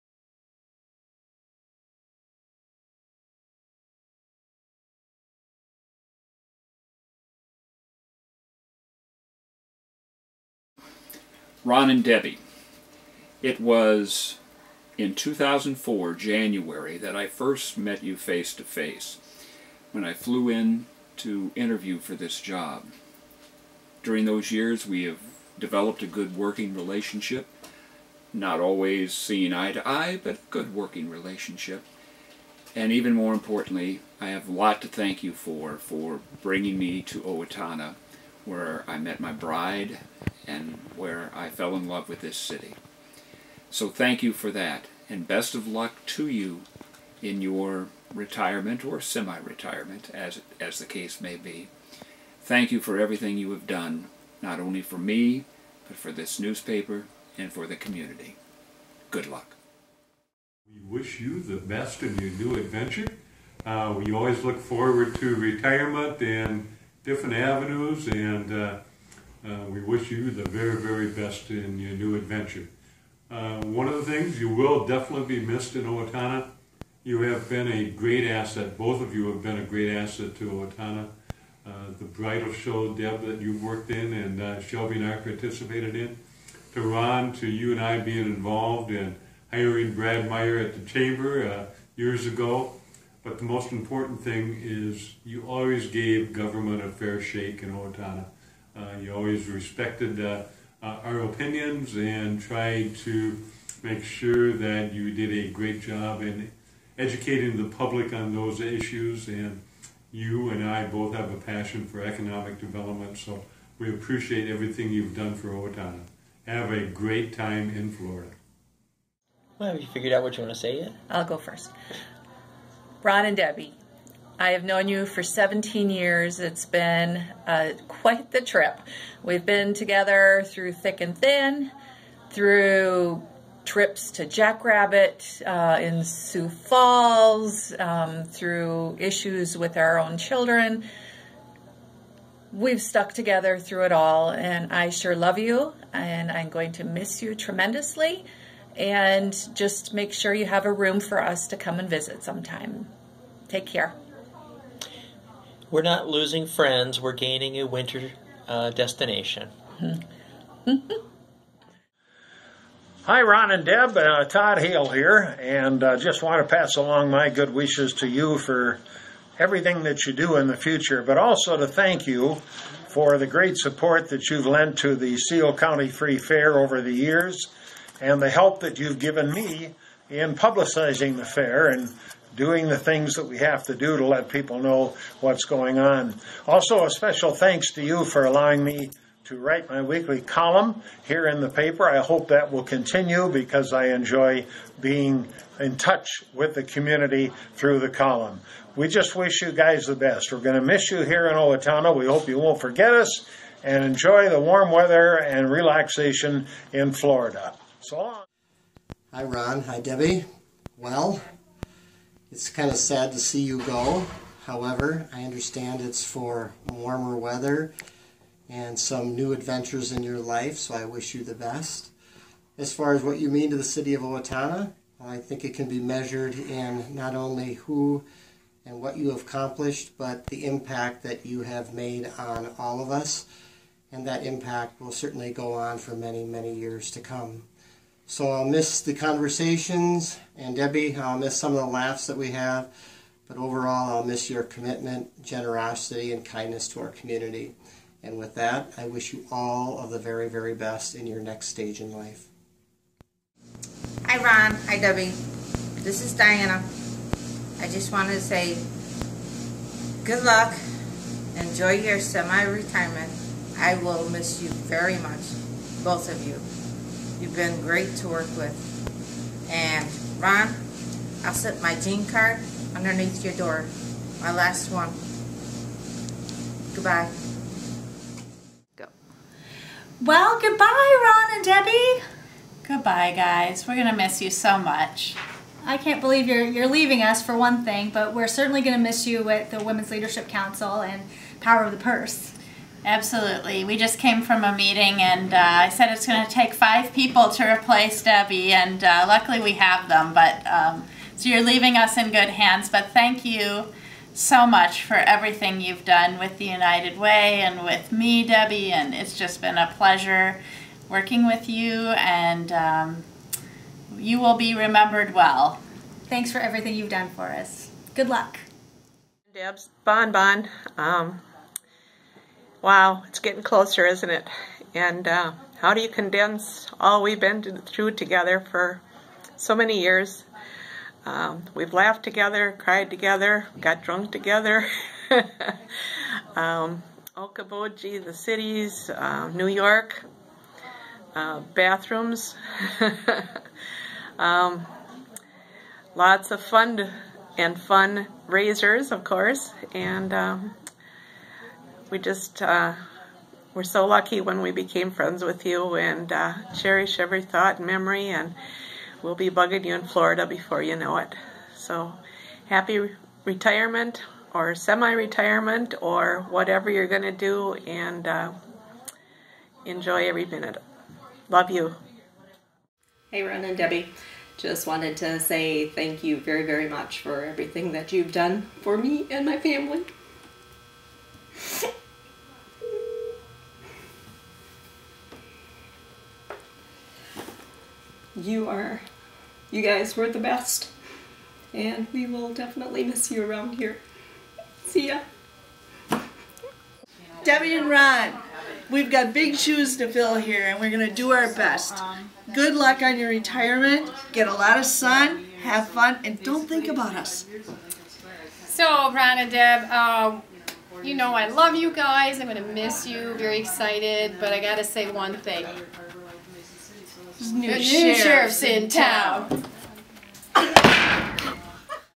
Ron and Debbie Ensley, Owatonna People's Press
A goodbye video for Owatonna People's Press publisher Ron and Debbie Ensley. Featured is OPP managing editor Jeffrey Jackson, Owatonna Mayor Tom Kuntz, Dave and Kim Purcell, Todd Hale, Police Chief Keith Hiller, OPP bookkeeper Dianna Schmidt, Tanya Paley and Kim Schaufenbuel of Steele County United Way, Owatonna Public Librarian Bonnie Krueger, OPP circulation manager Carol Harvey, and new OPP publisher Julie Frazier and OPP advertising director Ginney Bergerson. Video by Kim Hyatt, William Morris and Ashley Stewart, editing by Kim Hyatt.